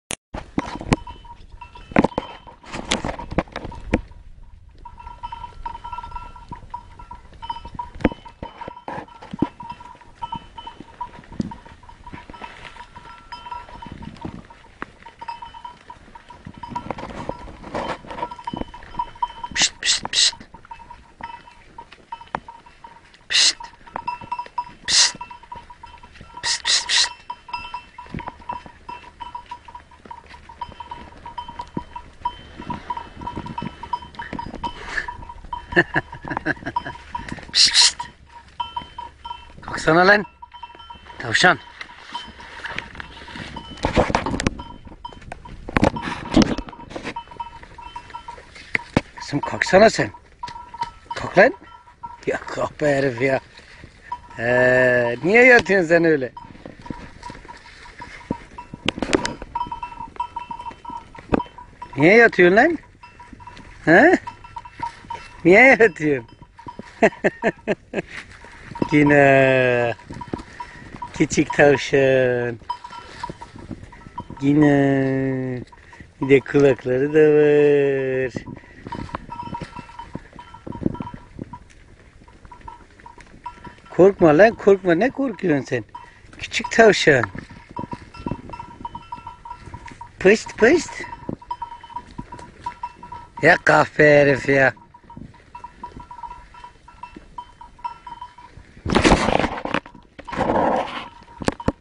hidden rabbit :)
hidden rabbit in wood.  Çalılıklarda gizlenen tavşan avı ve onu bulan çoban